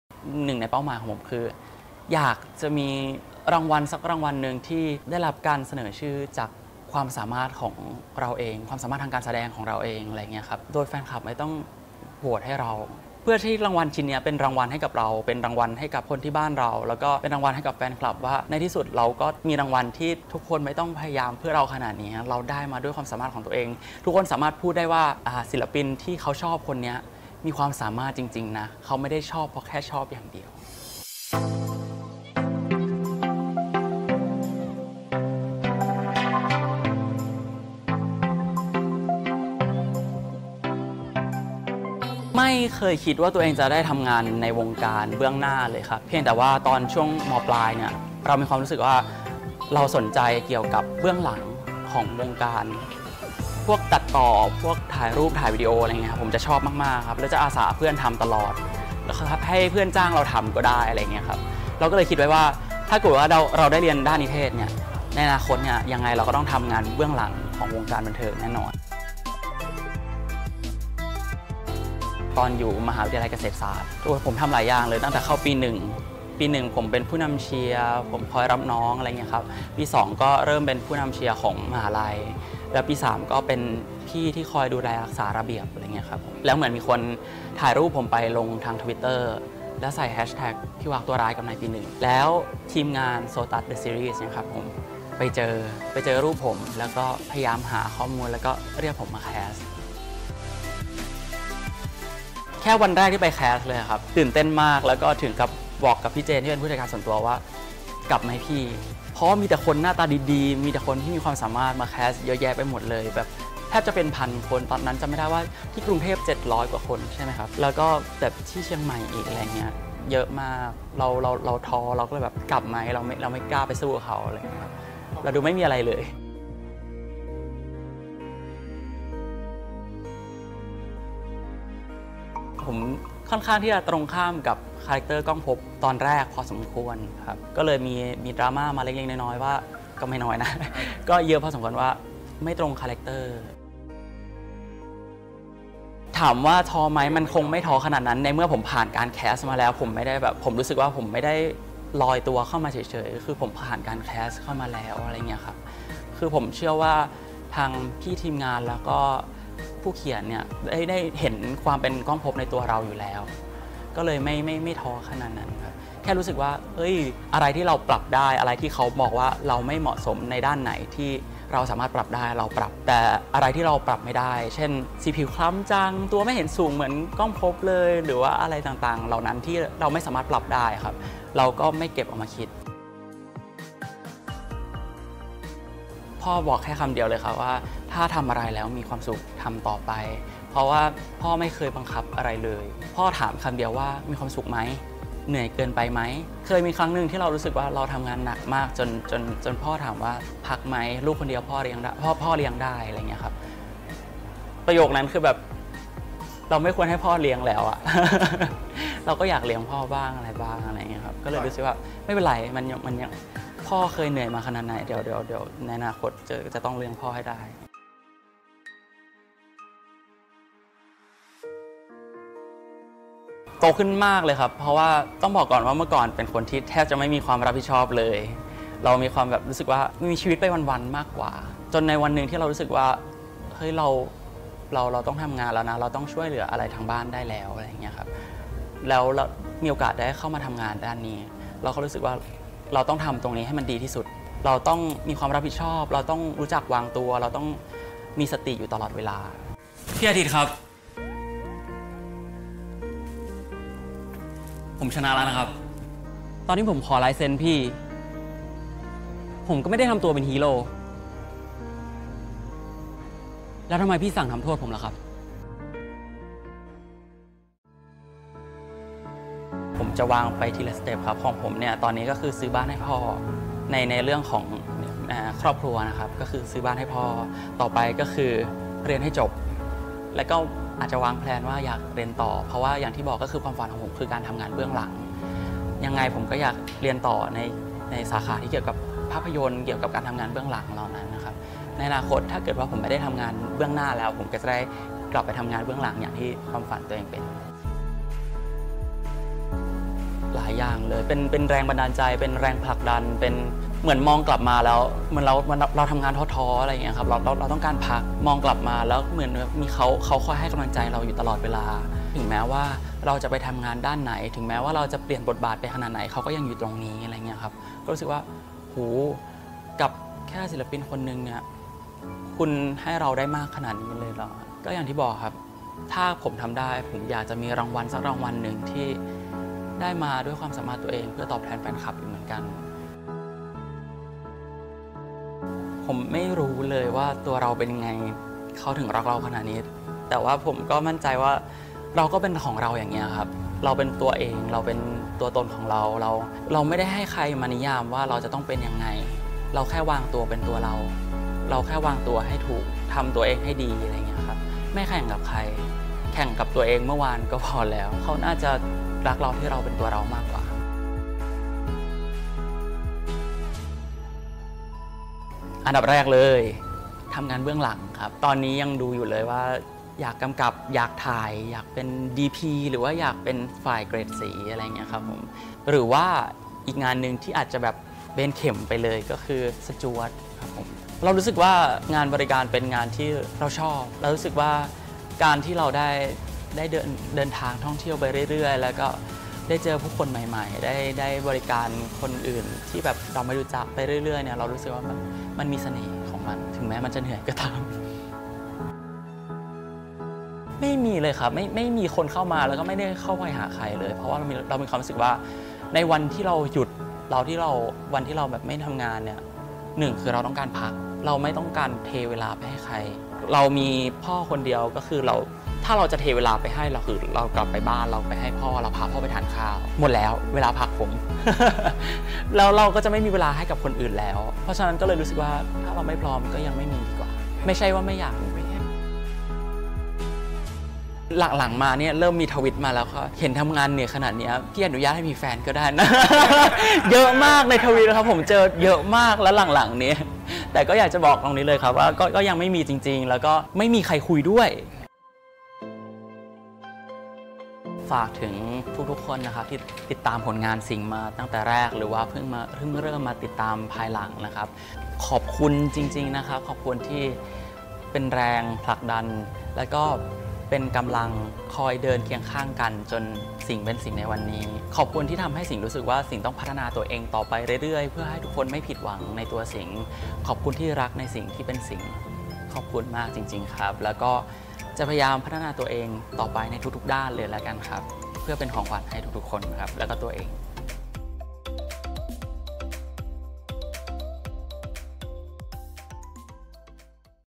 [EngSub] 180822 - Singto Prachaya's Interview with FEED
“Singto Prachaya Ruangroj” a playful handsome man who is well-known from “Sotus the Series”